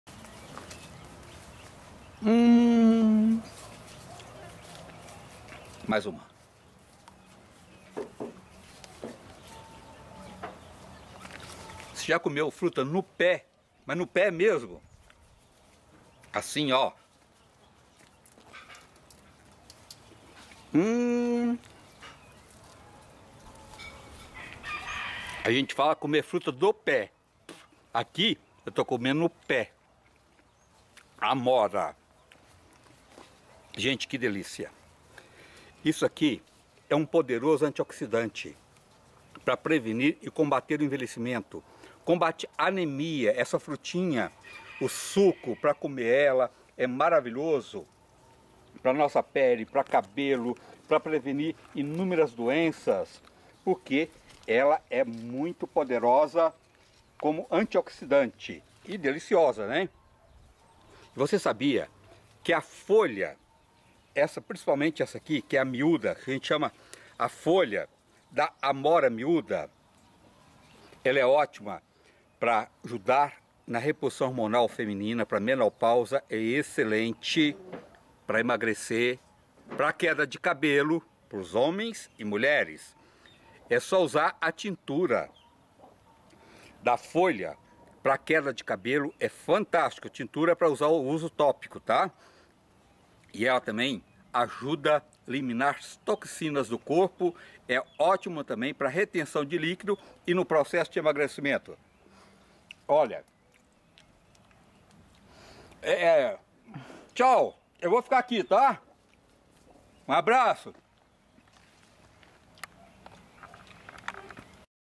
Farmácia da roça Amora
O Fitoterapeuta Andre Resende da dicas sobre o uso da amora.